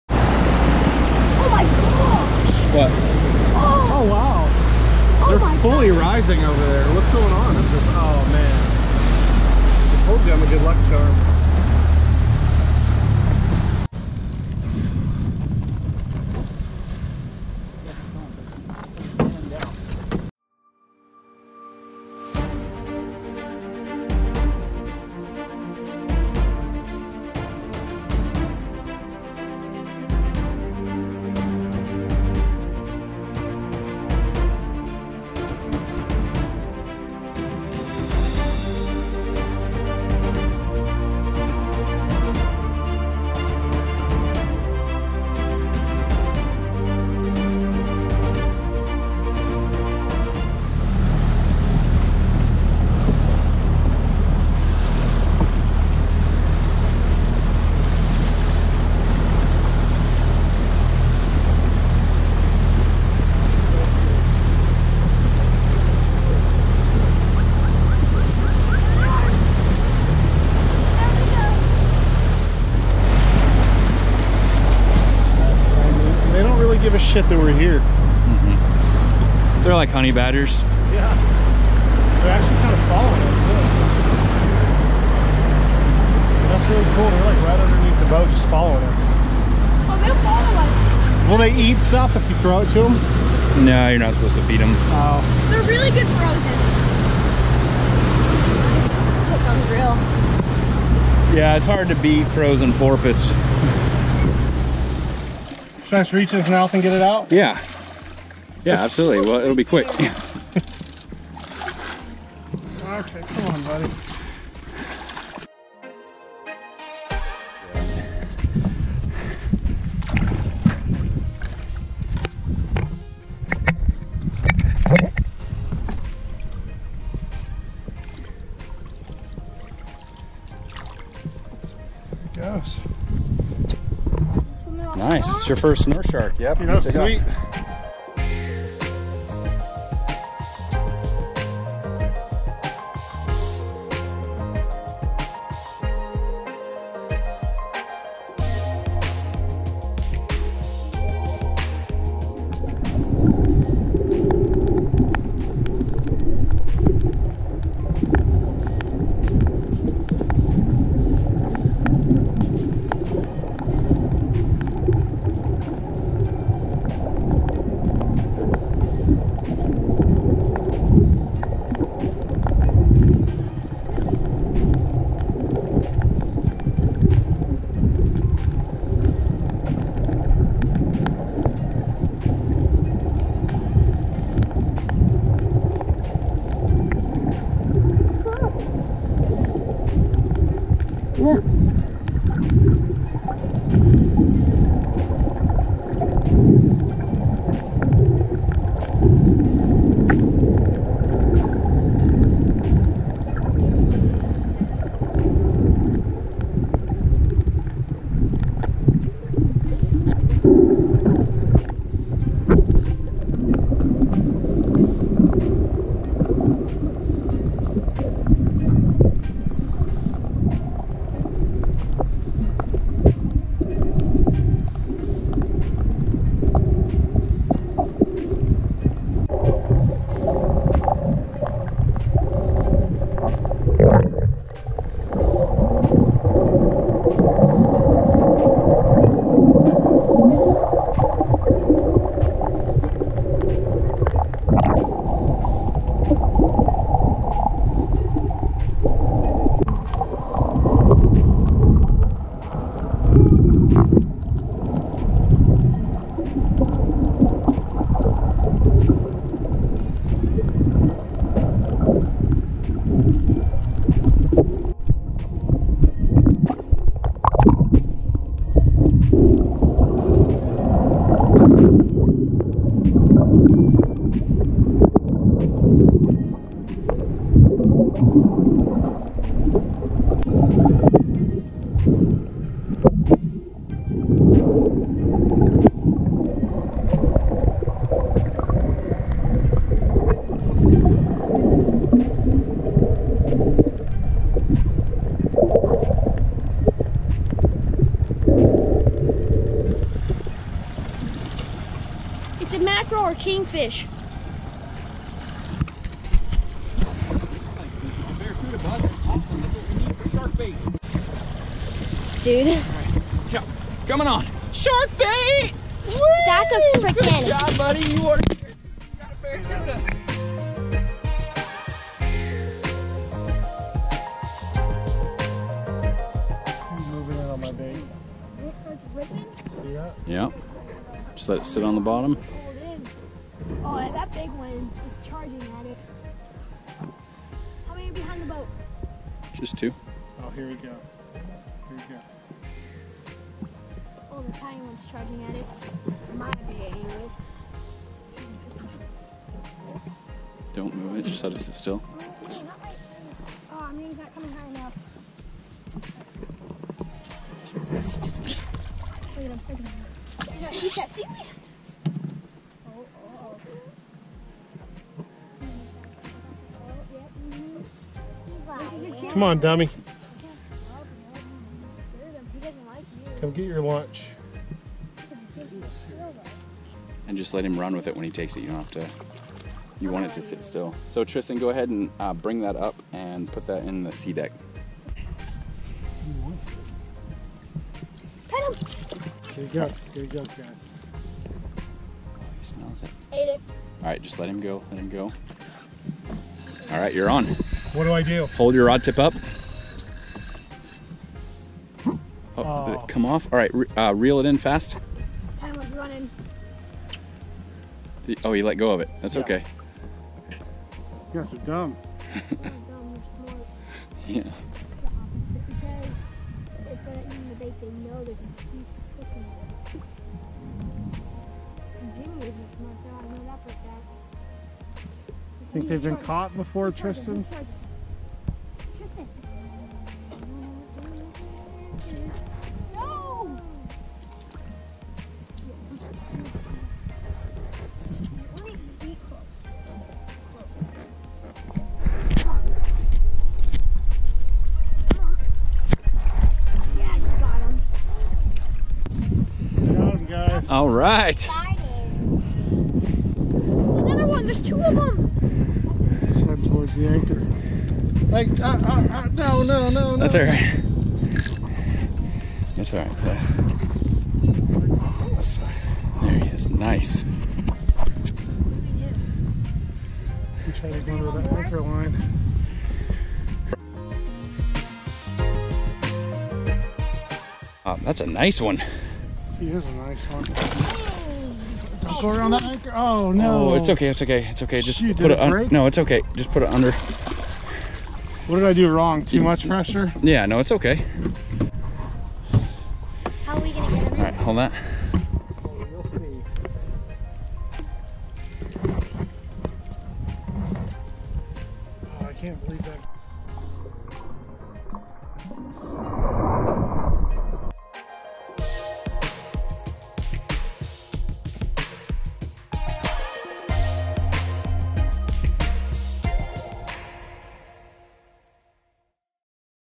The Key West Experience
Our buddy Jason from Colorado came to visit us in Key West.  He wanted to go snorkeling, see Dolphin/Porpoises, catch a Barracuda, a Shark, and to see a Manatee.  We were able accomplish it all!